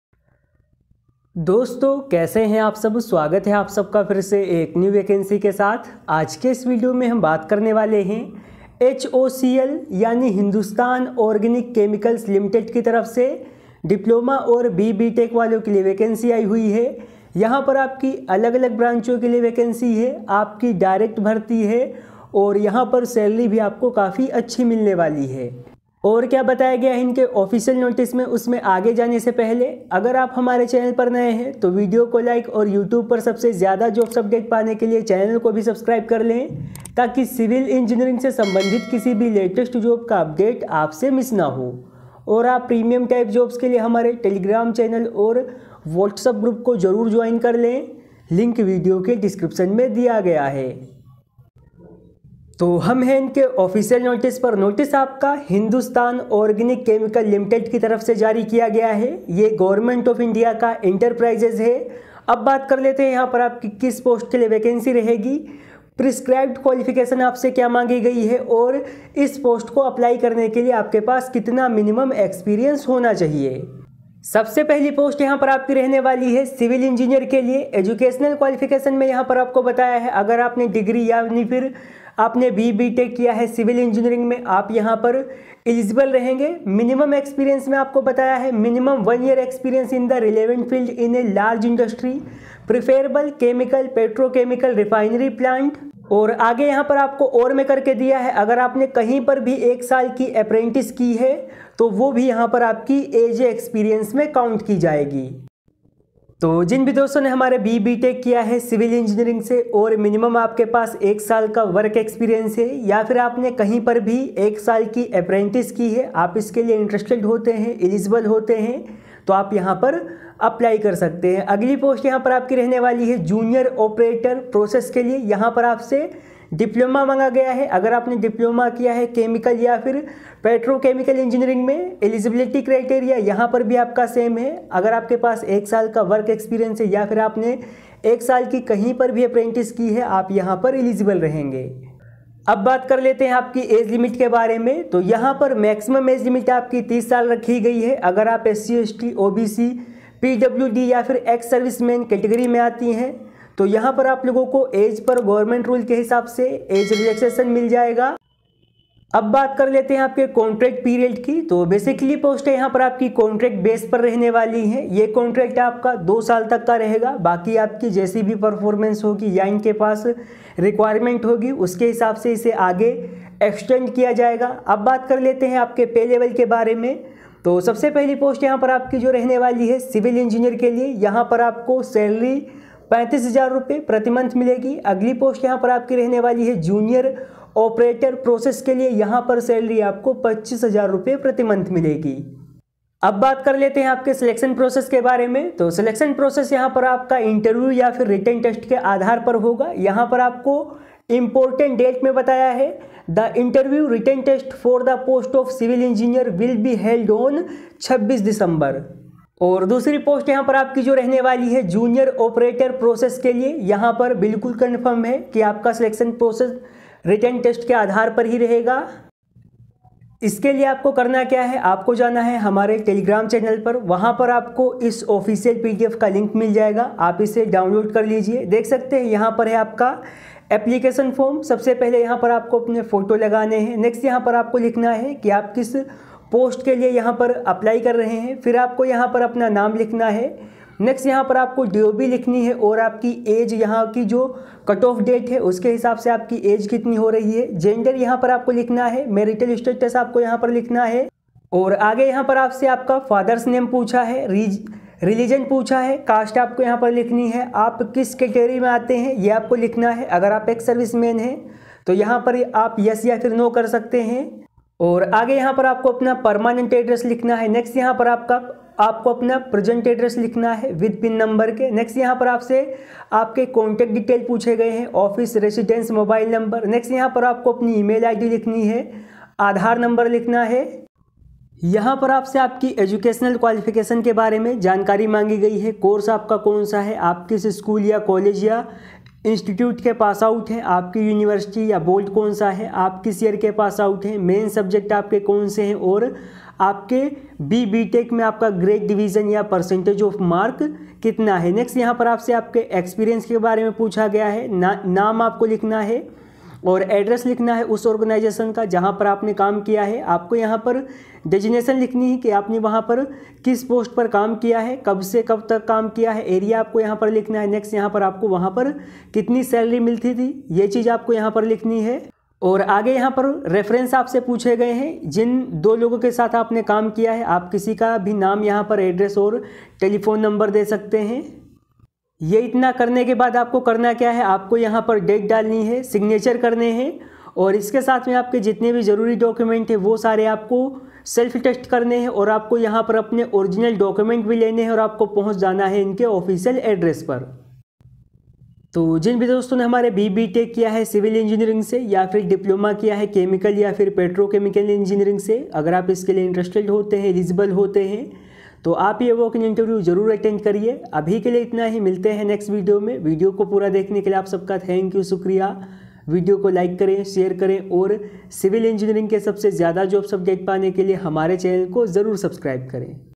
HOCL Recruitment 2022 Civil | Diploma, B.Tech | Salary ₹35,000 | HOCL Vacancy 2022
HOCL Recruitment 2022 Civil | Diploma, B.Tech | ₹35,000 | HOCL Vacancy 2022

✔️Walk in INTERVIEW⤵️⤵️️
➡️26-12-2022
Application Form Teligram चैनल से डाउनलोड करिये 🙏
🟡 आप सभी के प्यार और सपोर्ट के लिए तहेदिल से शुक्रिया... भगवान आप सबको खुशी, सेहत, समृद्धि, प्रगति और अमीरी दे... ❤😍🥰🙏

‎@Diploma Holders

❤Shear